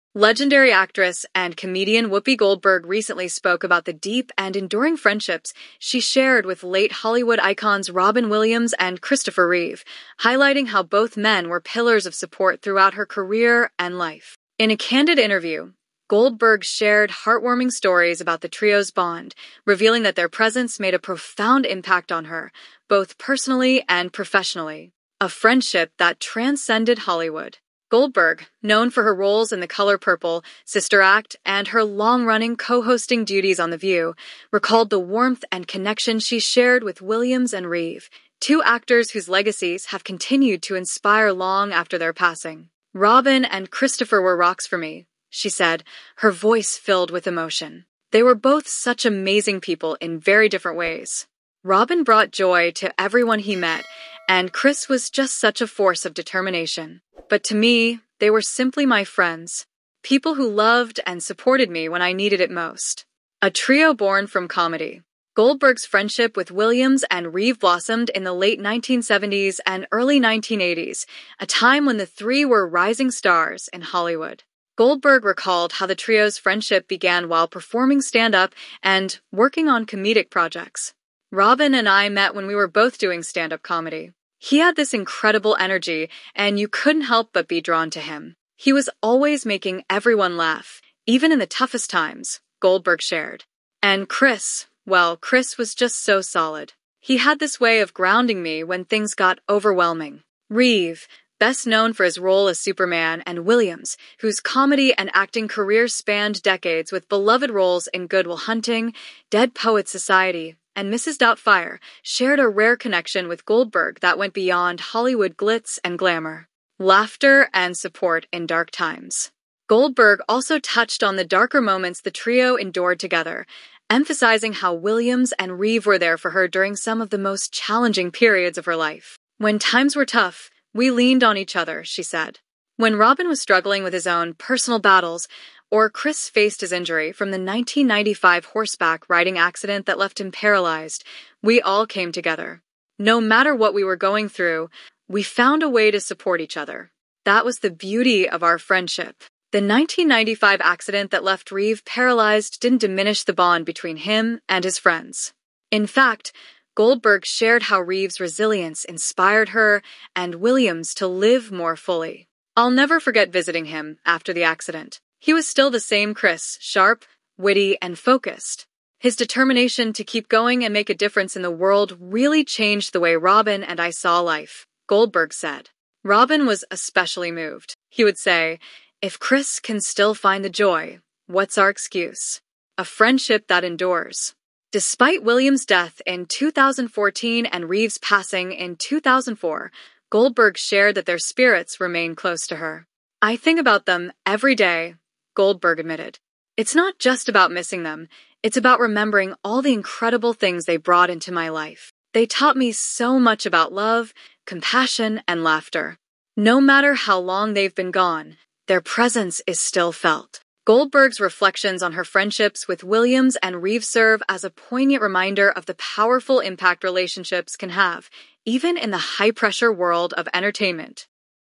Whoopi Goldberg Opens Up About Friendships with Robin Williams and Christopher Reeve They Were Rocks
Whoopi Goldberg Opens Up About Friendships with Robin Williams and Christopher Reeve: 'They Were Rocks for Me